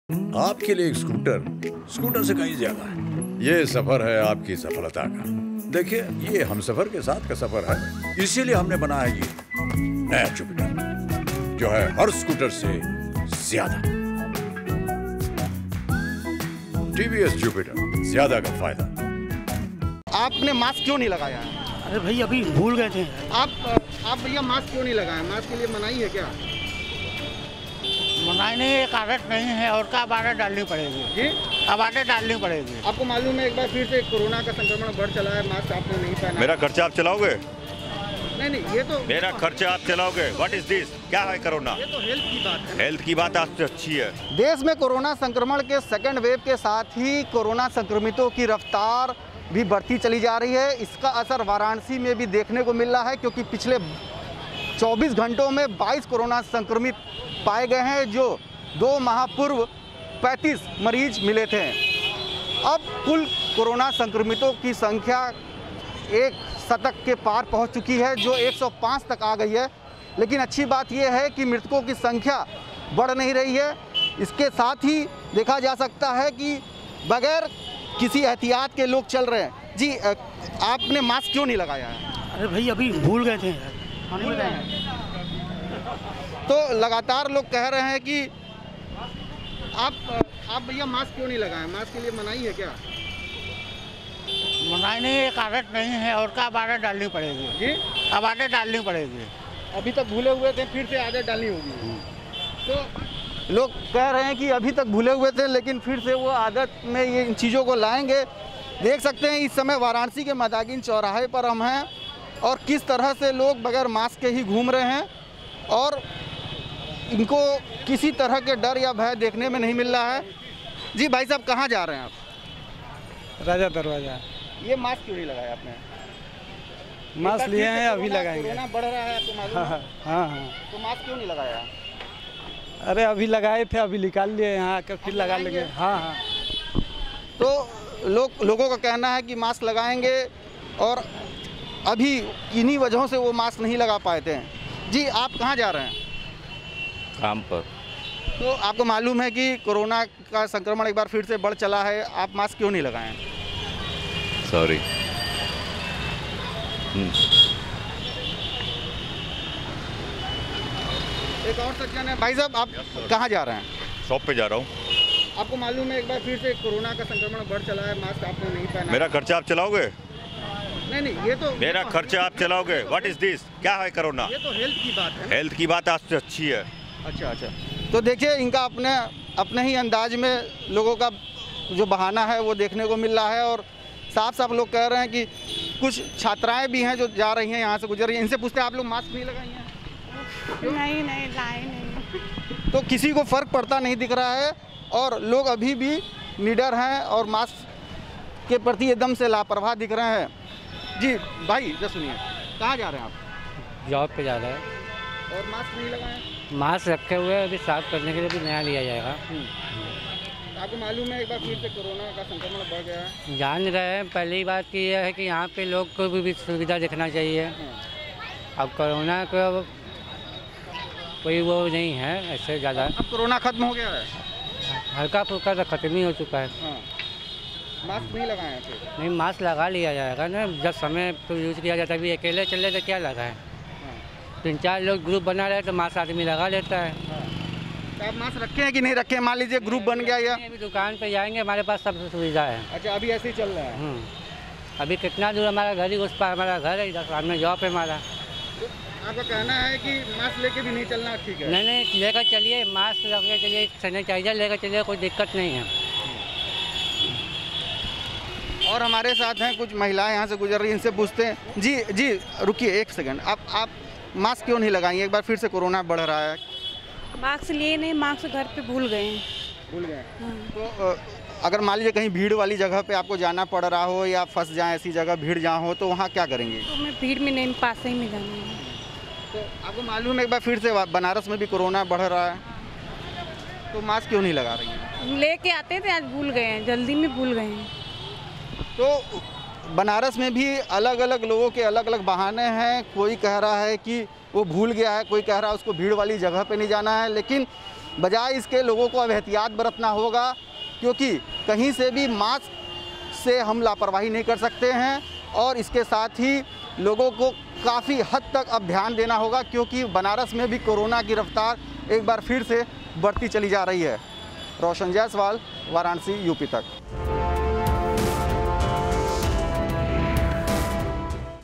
Reality Check: कोरोना के मामले बढ़ने के बाद भी लोग नहीं लगा रहे मास्क!
देश में एक बार फिर कोरोना के सेकंड वेव के साथ कोरोना संक्रमितों की संख्या फिर से बढ़ने लगी है। वाराणसी में 18 मार्च को रिकॉर्ड 22 कोरोना संक्रमित मिलने से खलबली मच चुकी है अब वाराणसी में कुल सक्रिय कोरोना संक्रमित मरीजों की संख्या एक शतक के पार यानी 105 तक जा पहुंची है।